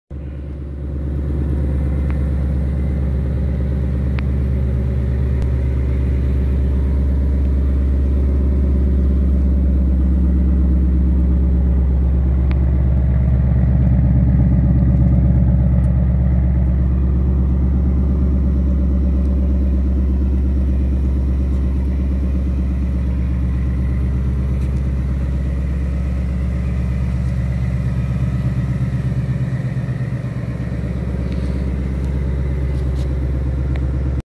2020 Mustang Paint Protected With Clear Bra Paint Protection Film and Coated By CarZmedics
2020 Mustang paint corrected and protected!
✅ Premium Shield Full Front Clear Bra PPF with custom stripe delete.
✅ REVIVIfy Pro Shield 10 year Coating

Learn everything you need to know about ceramic coatings and paint protection film...